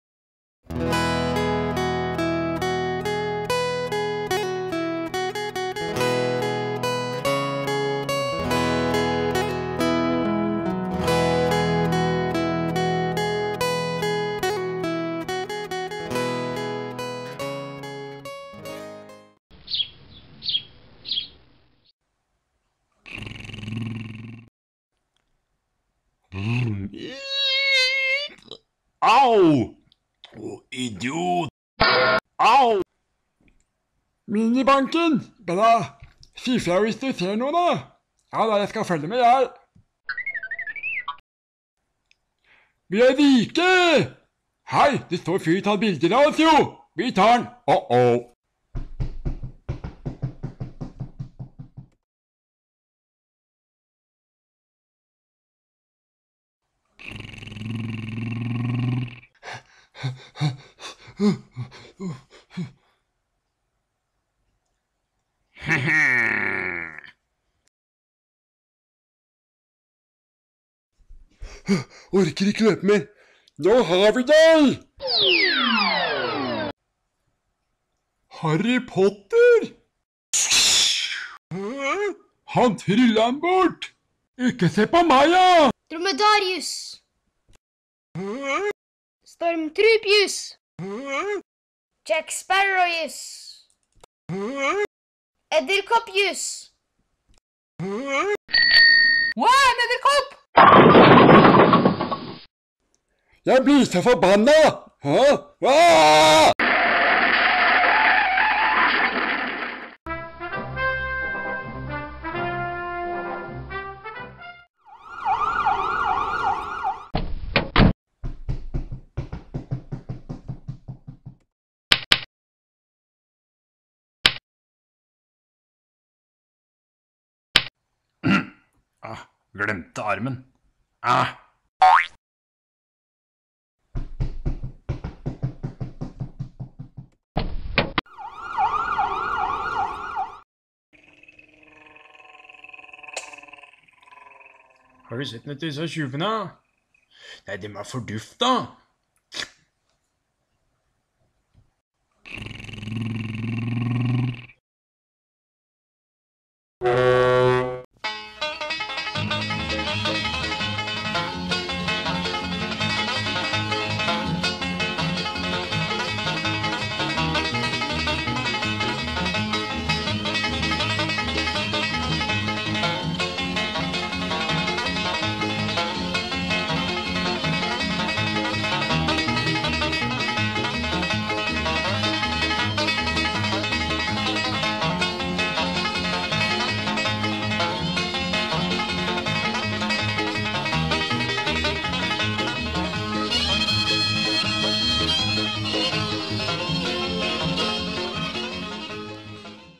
Minibank-ranet, en Lego-film av Andreas og Kristoffer
Vår andre animasjonsfilm med lego-figurer. Har innsett at lyd er alfa og omega, så denne gangen har vi lagt litt jobb i å få til et ok lydspor.

hilsen
Andreas og Kristoffer
Fjellhamar, Norge